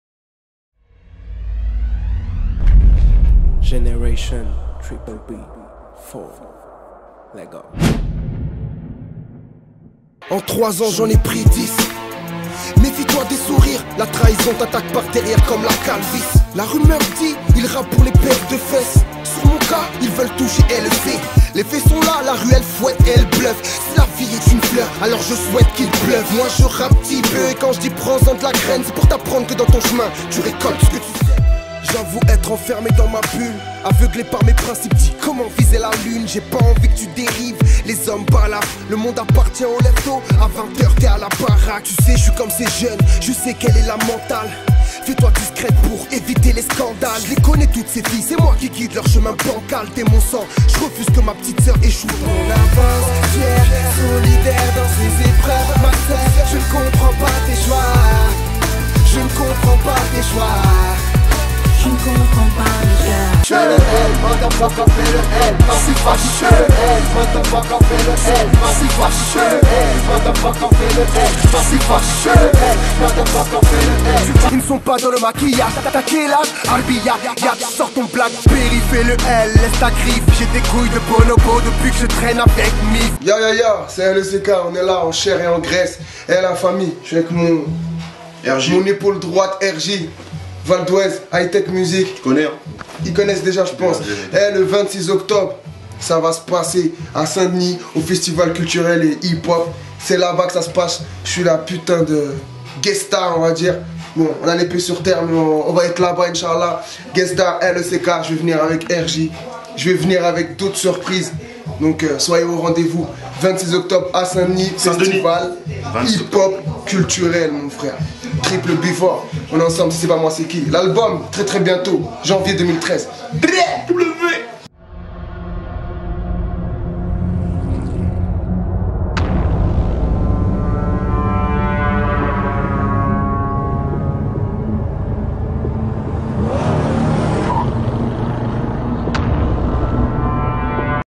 Annonce LECK au Show Génération Triple B 4 - 26 octobre 2012 à 20H à Saint-Denis
Lieu : Maison de la Jeunesse de Saint-Denis [Salle Ligne 13]
12 place de la Résistance et de la Déportation 93200 Saint-Denis
Métro : Saint-Denis Porte de Paris (ligne 13)

Vendredi 26 Octobre 2012 [20H - 00H]